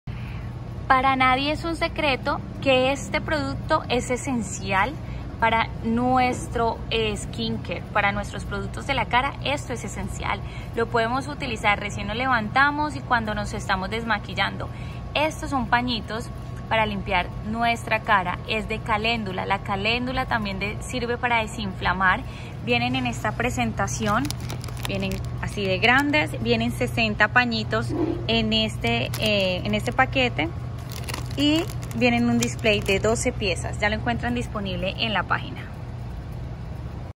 Cosmeticos Tejido de Limpieza Facial de Caléndula Moira Beauty l Cosmeticos al por Mayor
Venta al por mayor Moira Calendula Tejidos de limpieza facial (60 unidades) Acrílico contiene 12 paquetes Estas toallitas de limpieza facial eliminan todo el maquillaje sin la necesidad de enjuagar, perfecto para quienes están en movimiento.
Libre de crueldad | Vegano.

Para mas informacion por favor:

3390 NW 168TH ST.
MIAMI GARDENS,